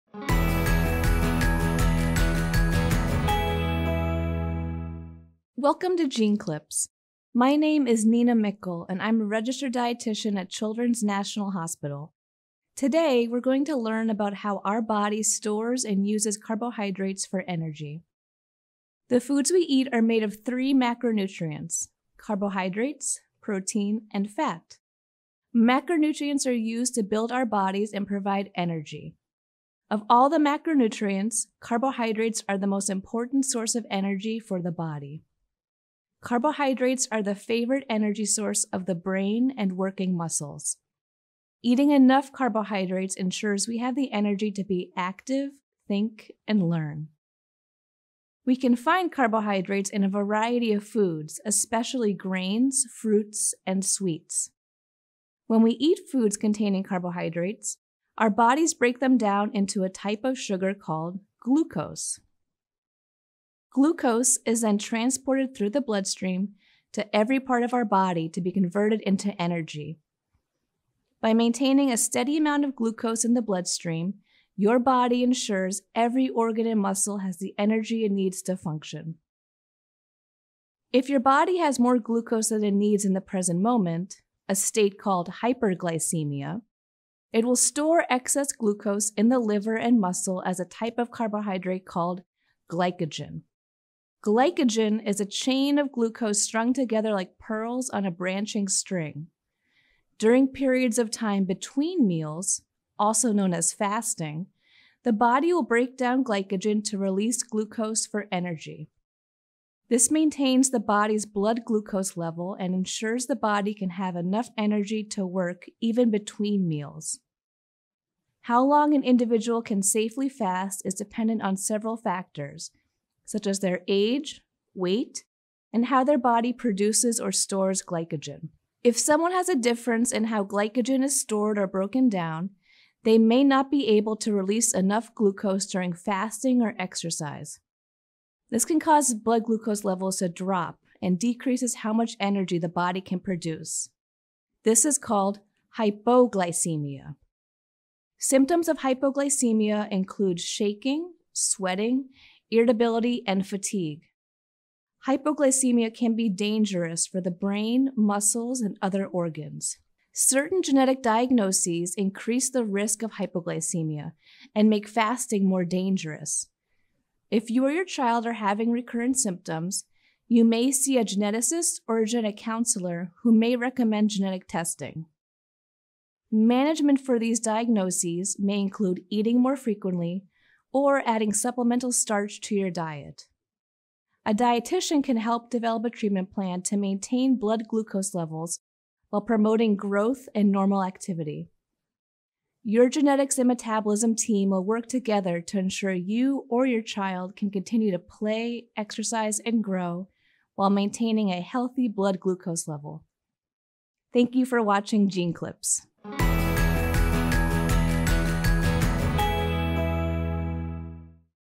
Carbohydrate Metabolism
Metabolic dietitian Nina Mickle describes how carbohydrates are broken down and used in our bodies.